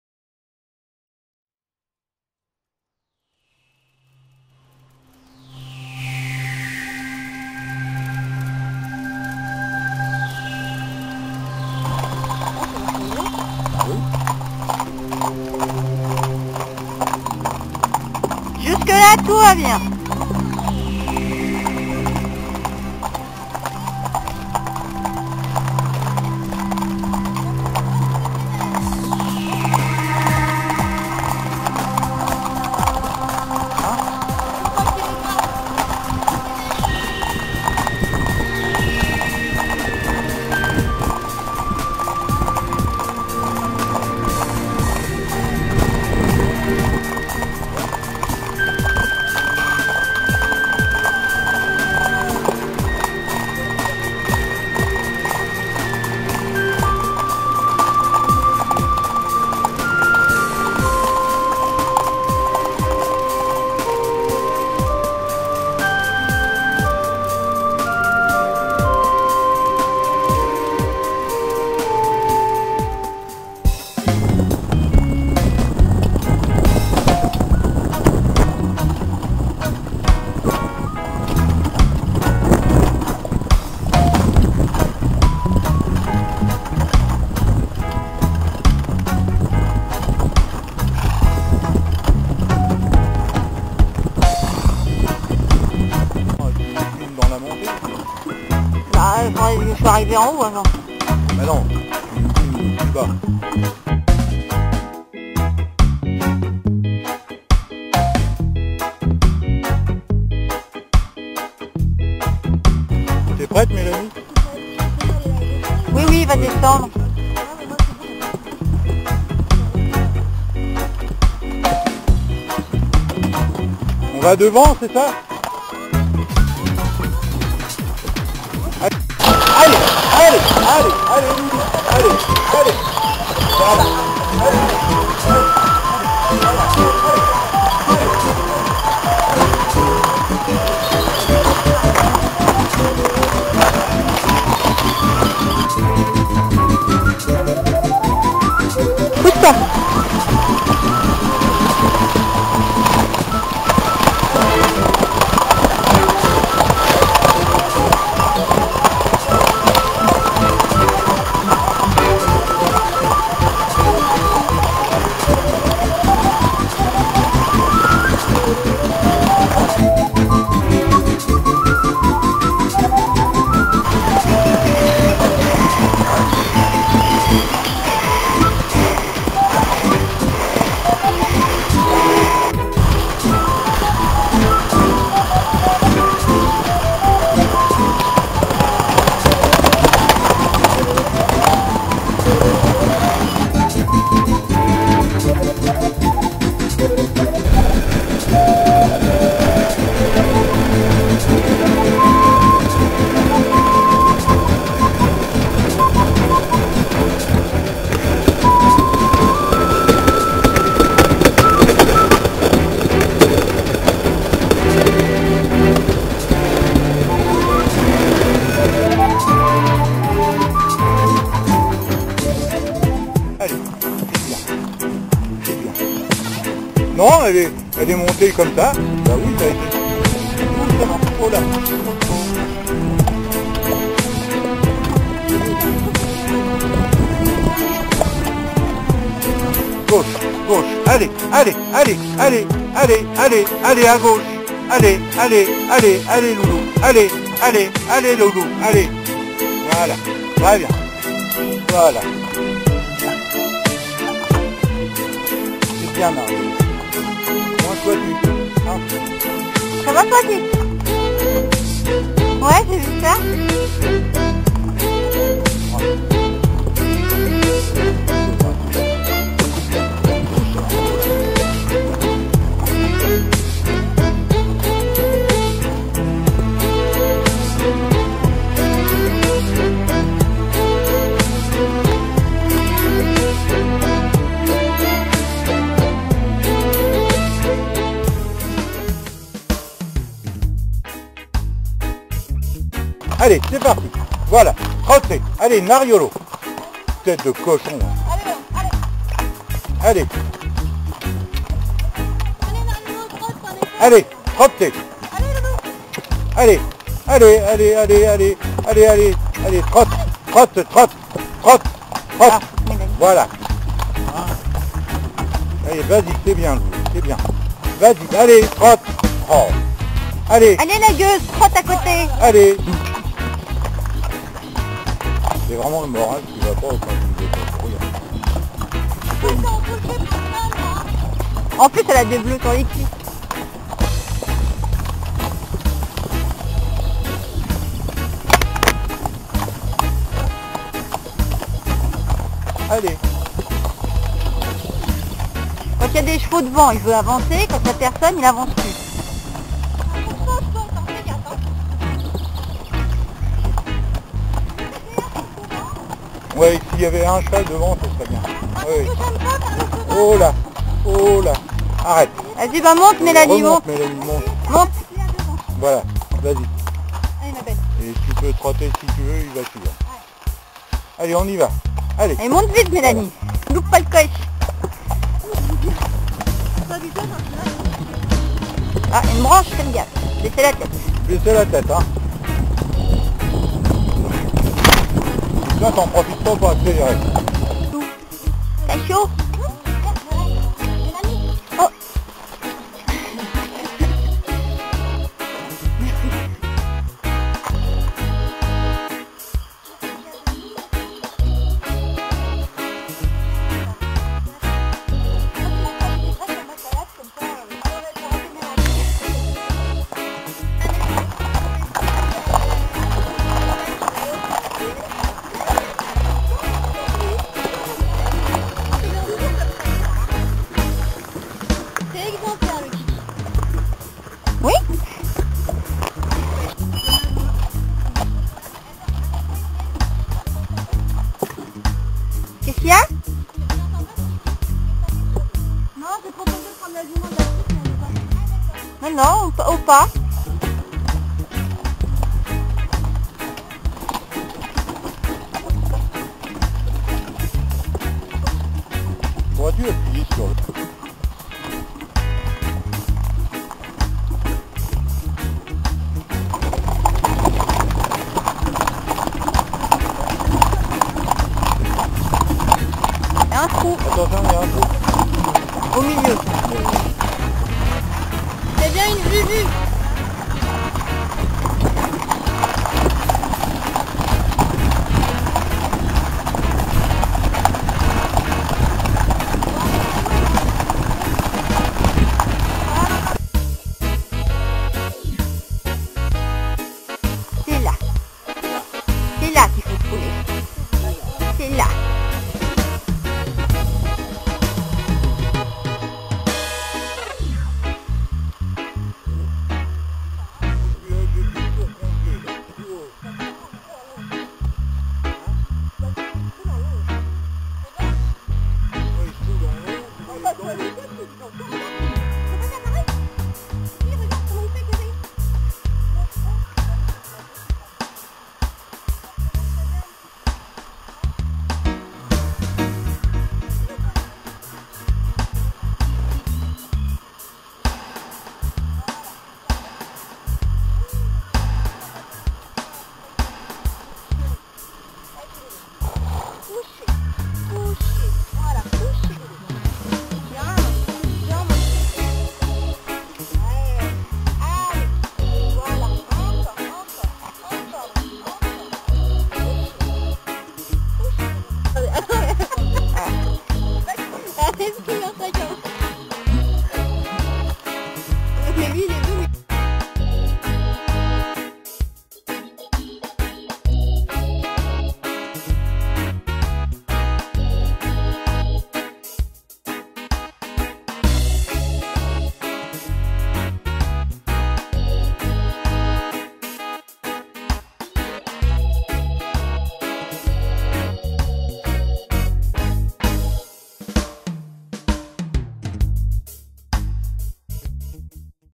2009-05-09 Monfort
Regroupement de chevaux provenant de SQP et balade en forêt de Monfort sur Risle.
Cette vidéo ne cherche pas à restituer les évènements mais à partager l'ambiance qui y régnait.

Merci à "Vénusia27" pour son acceuil et son organisation.